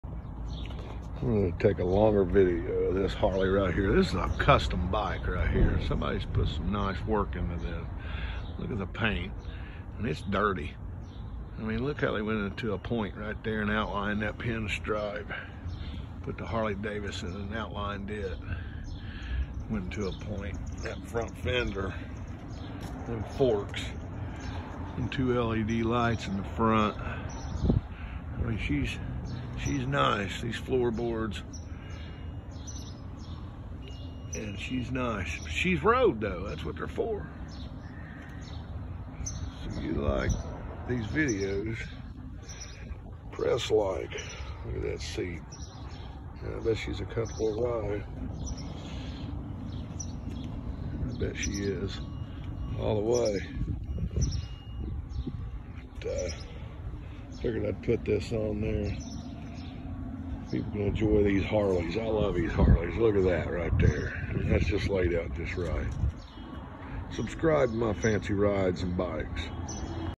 Sweet Yellow Harley Davidson @fancyrides5366
Nice custom Harley that gets rode has all the bells and whistles @fancyrides5366  beautiful bike